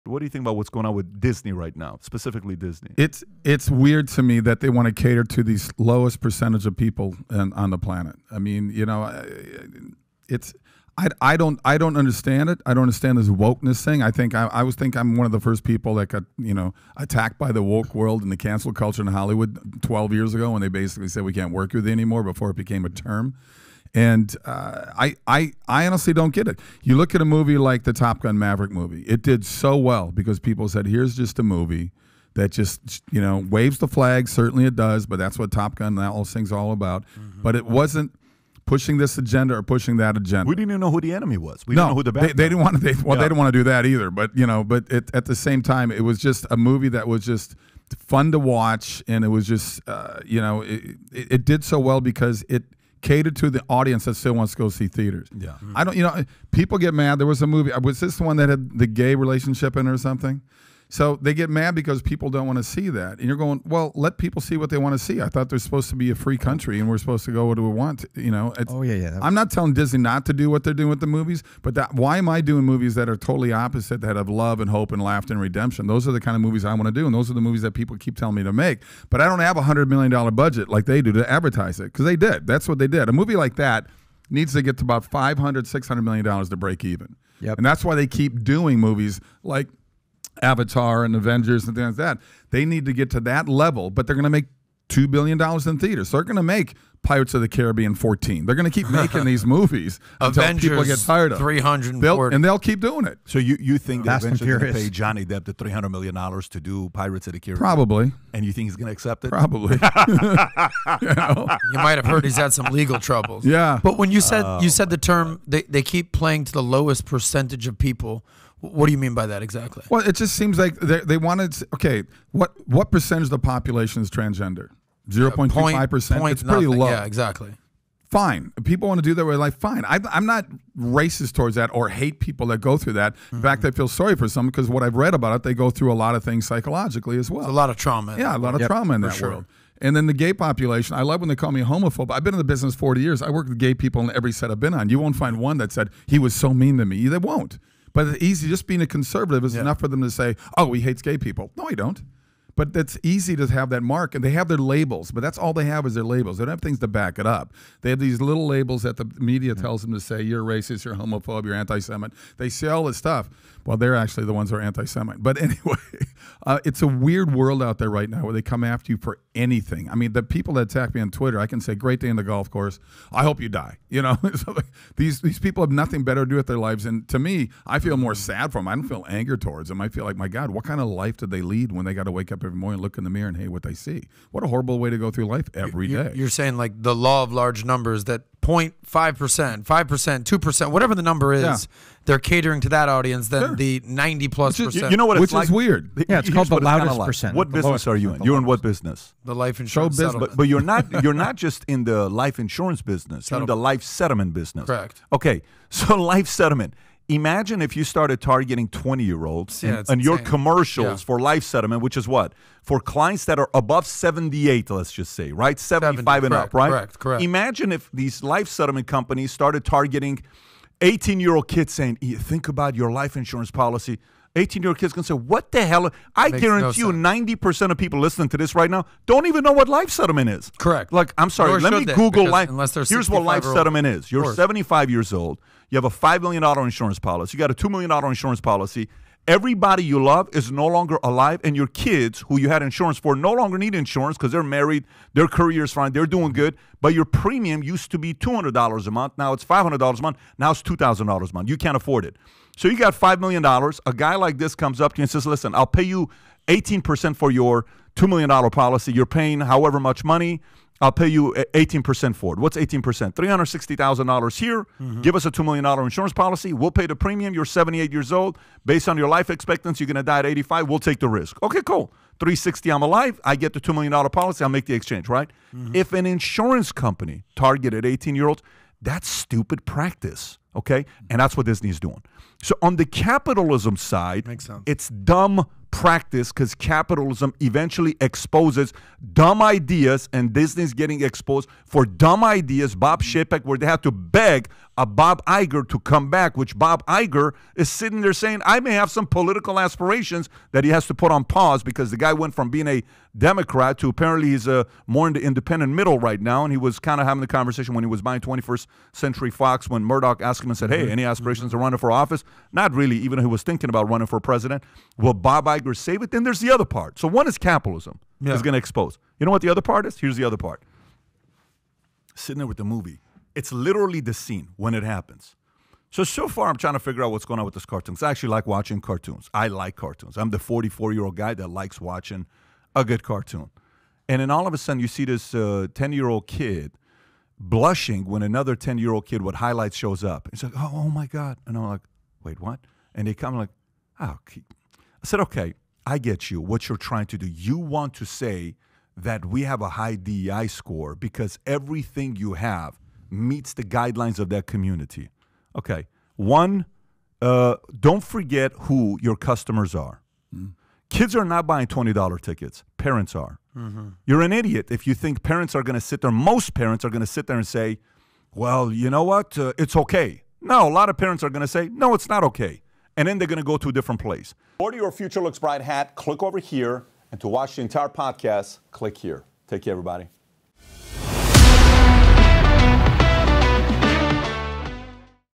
'They're Catering To The Smallest Percentage Possible!' - Reaction To Disney Going Woke
In this short clip, Patrick Bet-David, Kevin Sorbo, Tom Ellsworth & Adam Sosnick discuss Disney's wokeness.